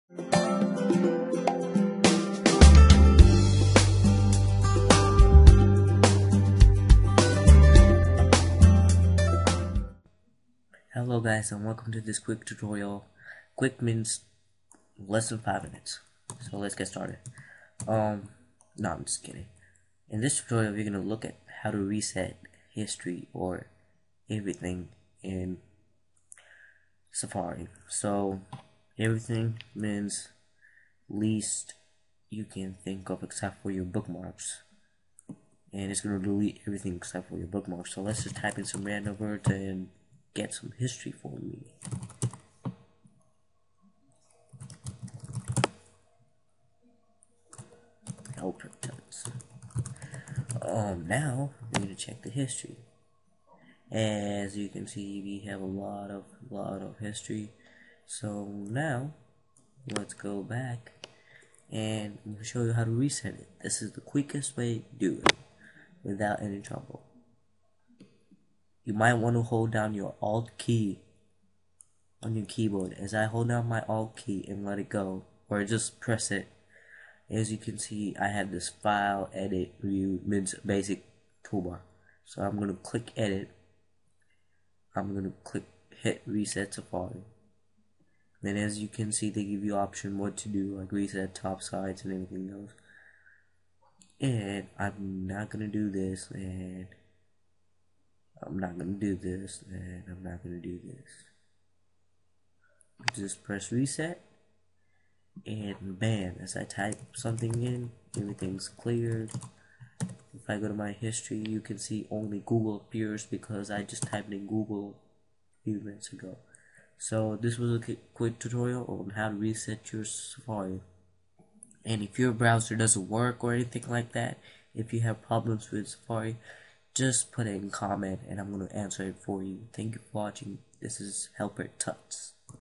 How to Reset Safari
In this quick tip Tutorial, we are going to look at how to reset safari. Reseting Safari basically Reset the history, password, Top Site and etc... Depending what you choose in the setting pop up box.